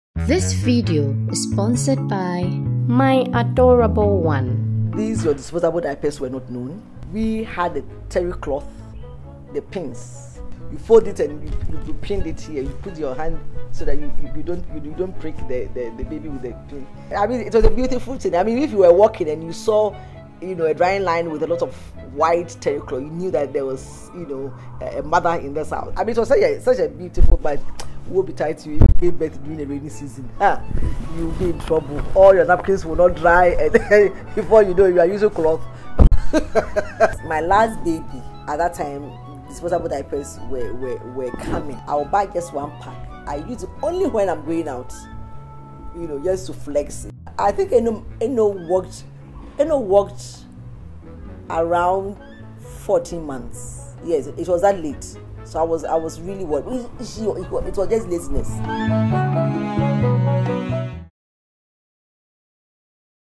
WE DIDN'T HAVE DISPOSABLE DIAPERS IN OUR TIME _ Mrs Asenso Okyere _ Trailer
COMING UP NEXT :
Today on GrannyTales is Mrs. Nana Yaa Asenso-Okyere who turns SIXTY today!!
She's the mother of Eno of 'Talkative Mom' blog, and also has two grandchildren.

We are a team that's dedicated to telling the Ghanaian stories surrounding pregnancy, childbirth and parenting.